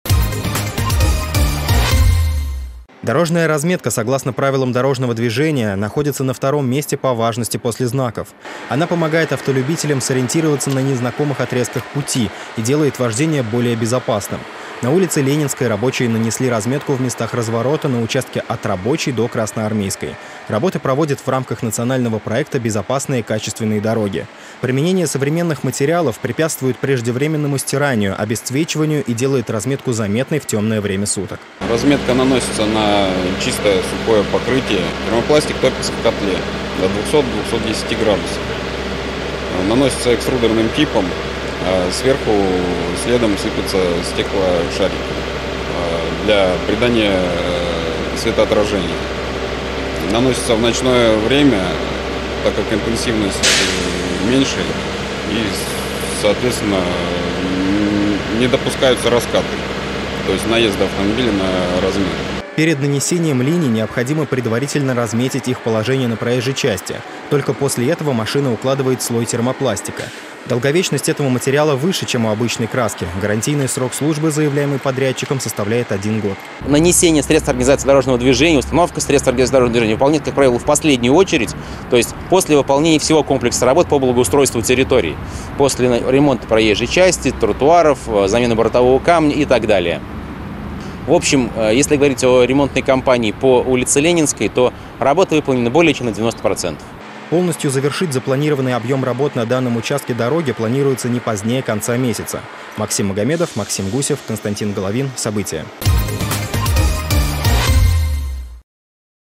В Самаре на обновленной улице Ленинской наносят разметку
Ну улице Ленинской ночные художники нанесли дорожную разметку. Любой автомобилист сталкивается с ней ежедневно, но как наносят эти линии знают не все. Максим Магомедов познакомился с технологией.